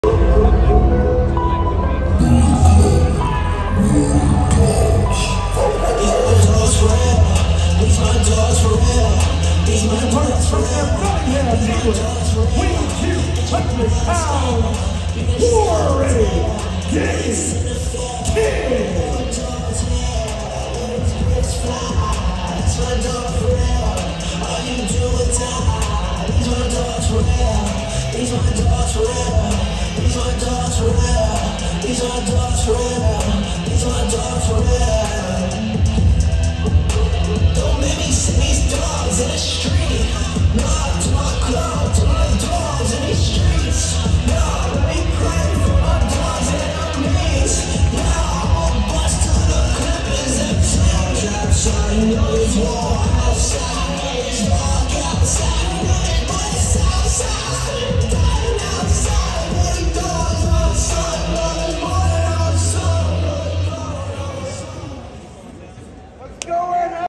Gabe Kidd entrance live - Ring of Honor taping 11/07/2024
NJPW Strong Openweight Champion Gabe Kidd makes his entrance live during the Ring of Honor taping